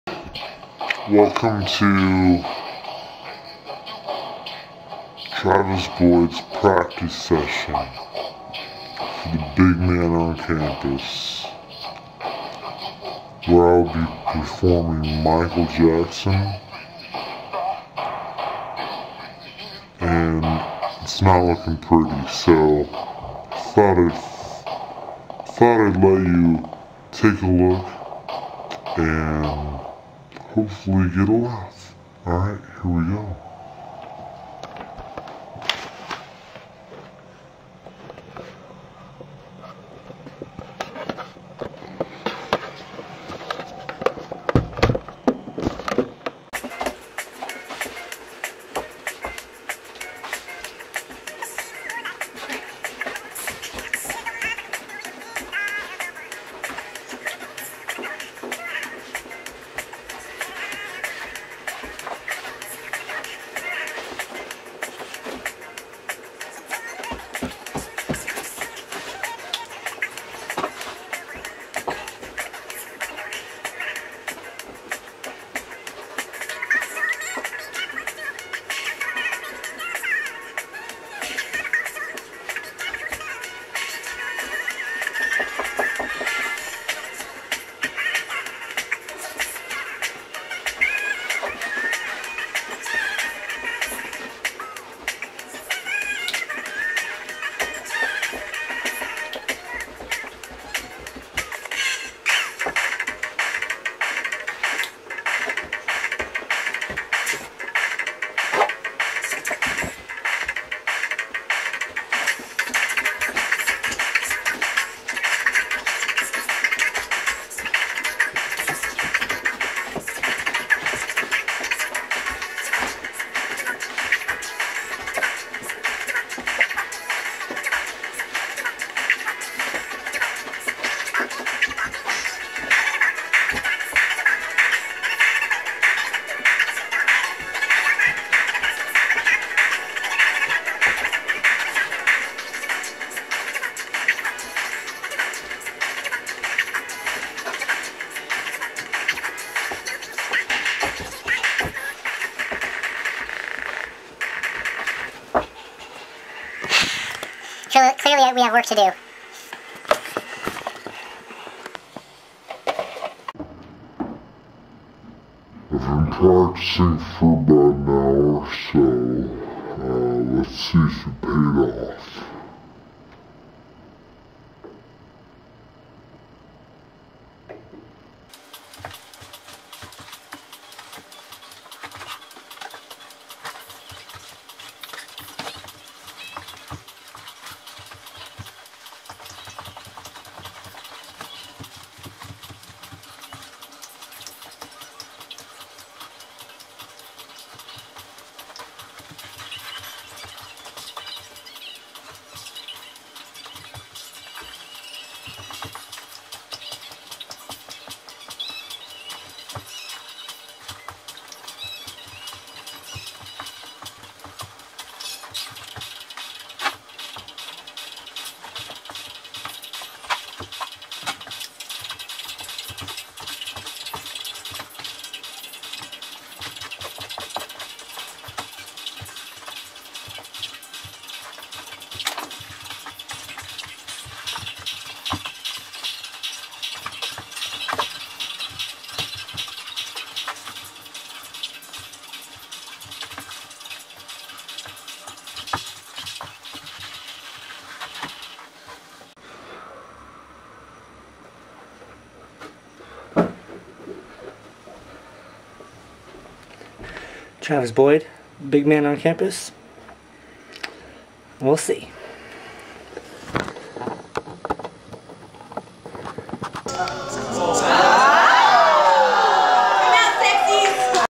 Travis Boyd's Practice Session for Big Man on Campus
Travis doing some dance moves